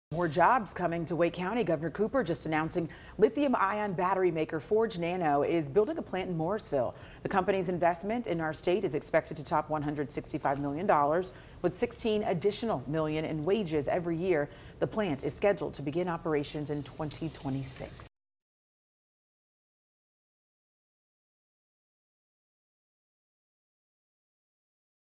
Battery manufacturing facility to bring hundreds of high-paying jobs to Morrisville
Forge Battery expects the lithium-ion manufacturing facility to be operational in 2026.